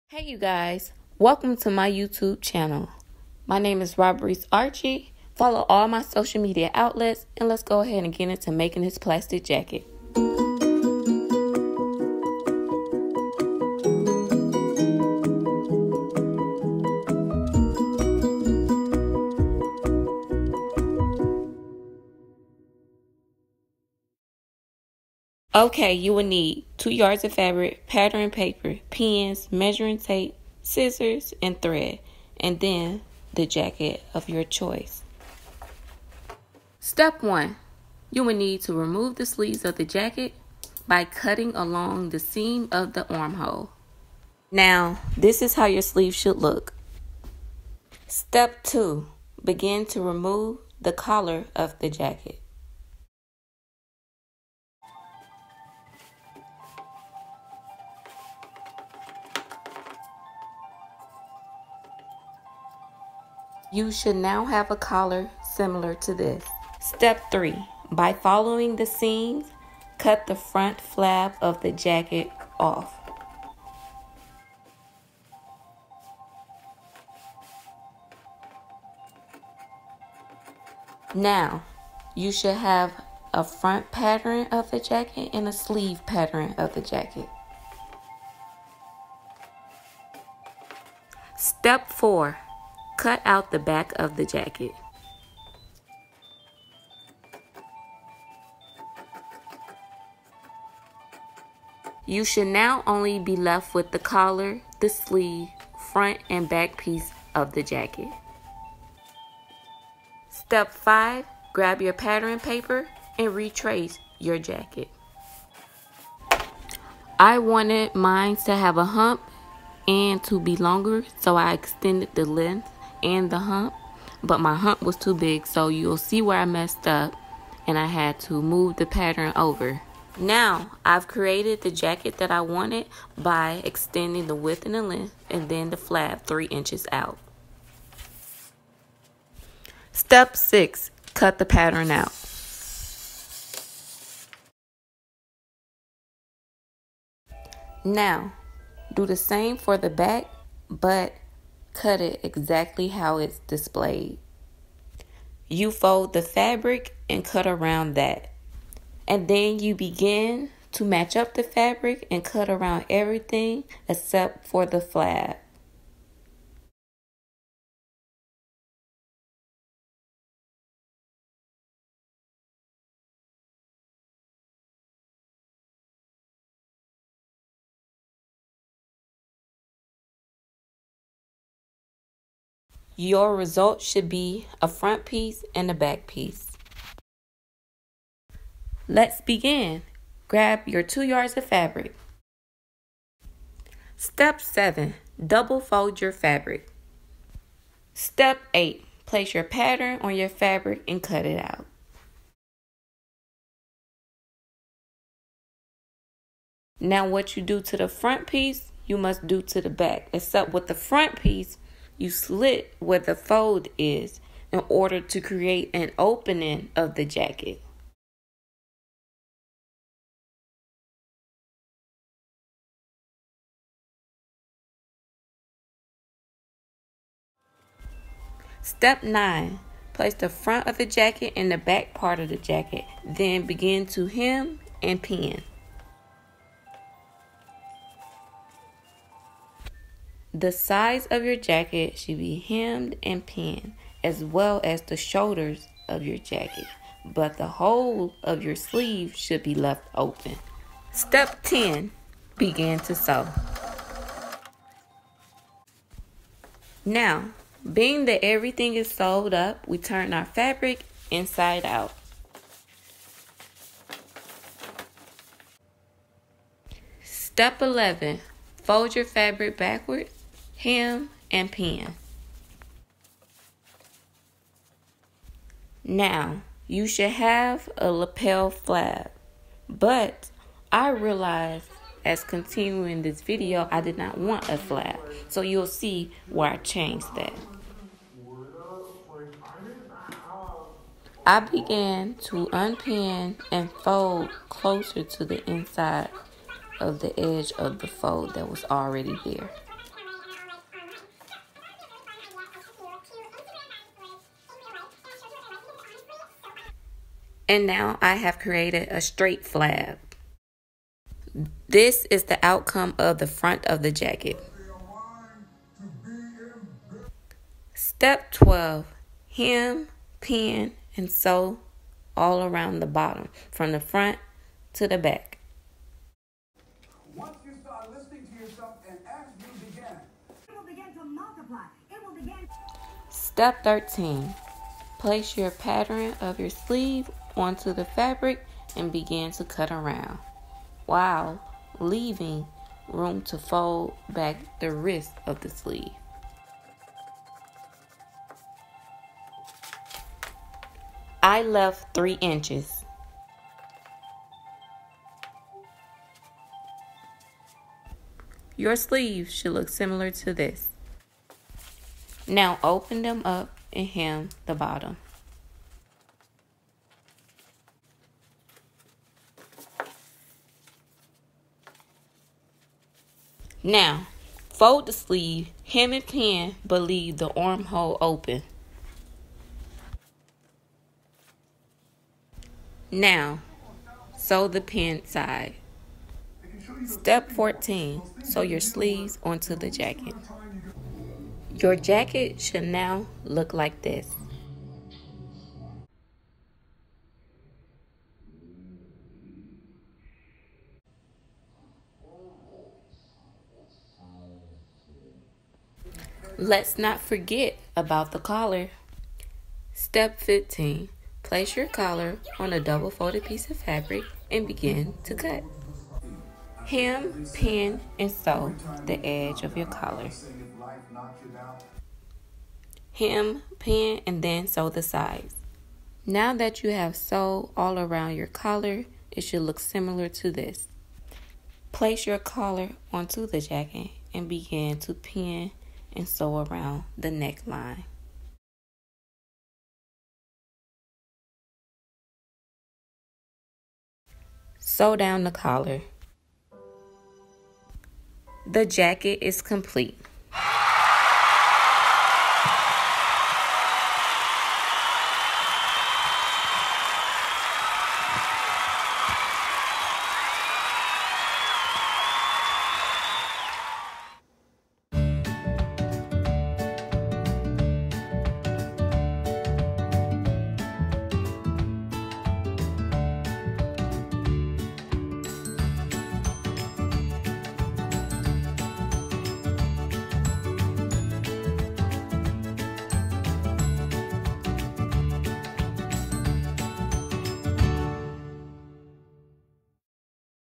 How to Sew a Jacket Sewing a Clear| Plastic | Vinly Jacket
Sewing a Clear Red Vinly (Plastic) Jacket |How to Sew
Handmade by Robrice Archie

Supplies Needed : Pins , Thread , Scissors, 2 yards of Fabric , Pattern Paper , a Jacket of your Choice

Purchase Jacket: Excited to share this item from my etsy shop: Red Plastic Jacket /Clear Coat

Available in most colors requested.

Copyright :
I own no rights to music in this video.